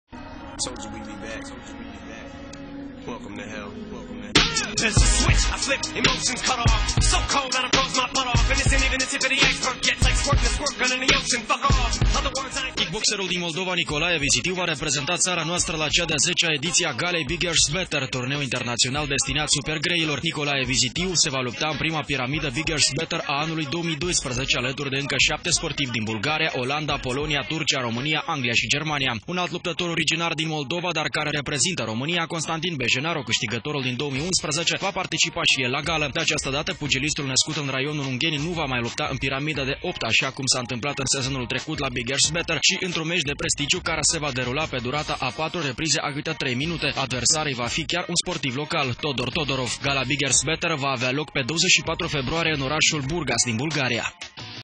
Încă un unghenean va lupta în prima piramidă Biggers Better
Luptătorul unghenean Nicolae Vizitiu va participa la turneul internaţional de box destinat super-greilor Biggers Better. Vizitiu, care practică kickboxingul, se va duela cu cei mai buni luptători din şapte ţări. Vizitiu nu va fi însă singurul unghenean la această competiţie. Constantin Bejenaru, originar din Ungheni, dar care reprezintă România, va lupta într-un meci de prestigiu.